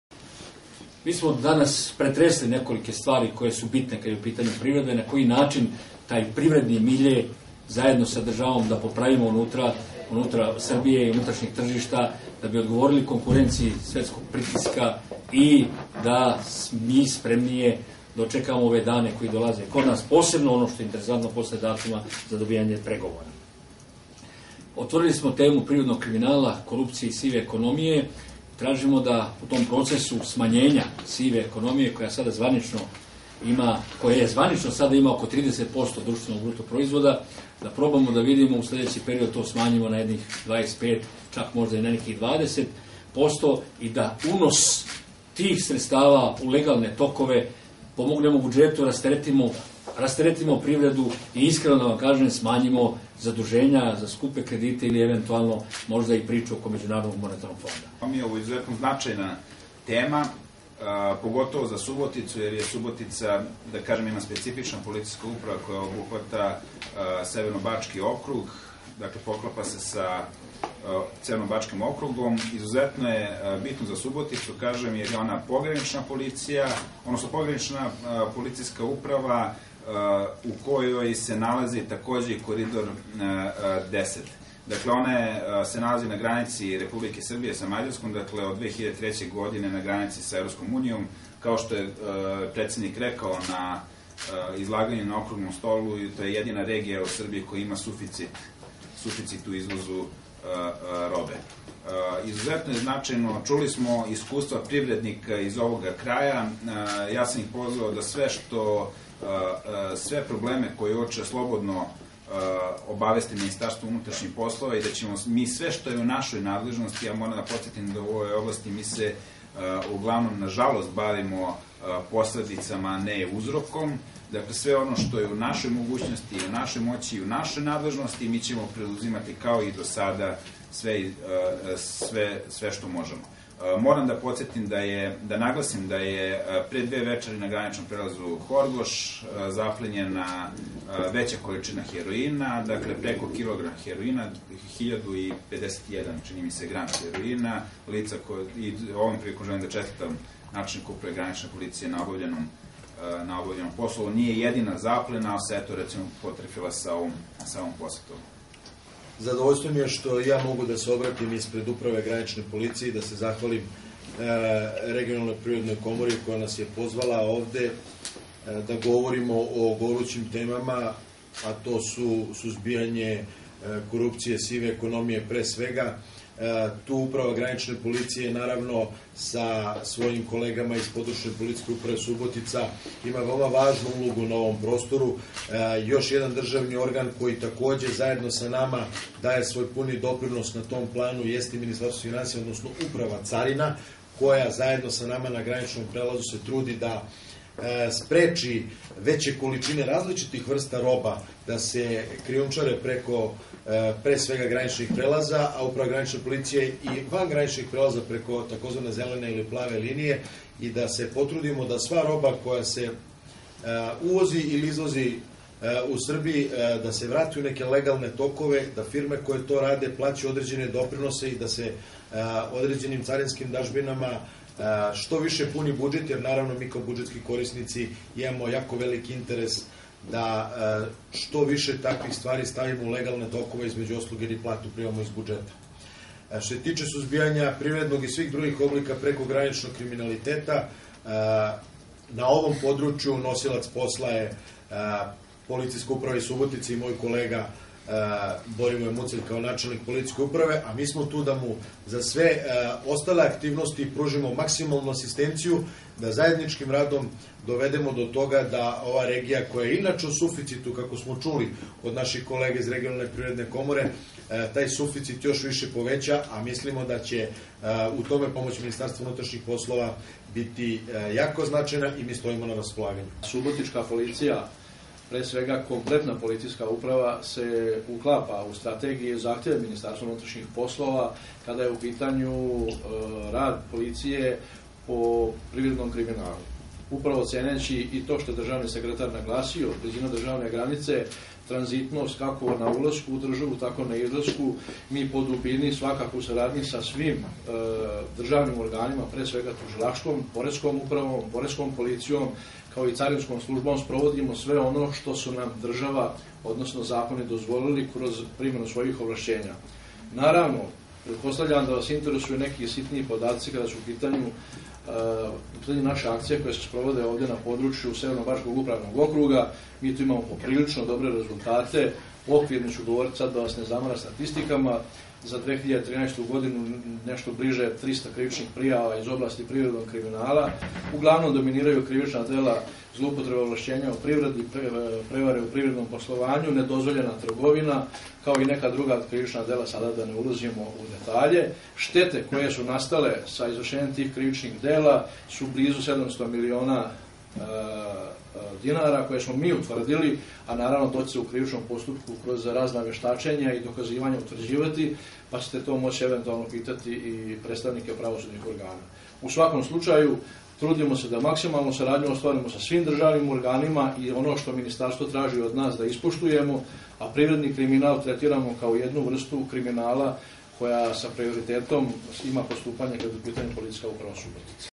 okrugli sto o suzbijanju sive ekonomije i privrednog kriminala
super-info.rs

Neophodno smanjenje sive ekonomije i privrednog kriminala

Subotica, 10. marta - U Regionalnoj privrednoj komori održan je okrugli sto na temu suzbijanja sive ekonomije i privrednog kriminala, kom su prisustvovali  državni sekretar u Ministarstvu unutrašnjih poslova, Vanja Vukić i državni sekretar u Ministarstvu finansija, Ljubiša Milosavljević.

Siva ekonomija predstavlja ozbiljan problem u privredi s obzirom da obuhvata čak 30 % društvenog bruto proizvoda, a cilj današnjeg skupa bio je iznalaženje načina da se ovako visok procenat smanji na prihvatljivu razinu.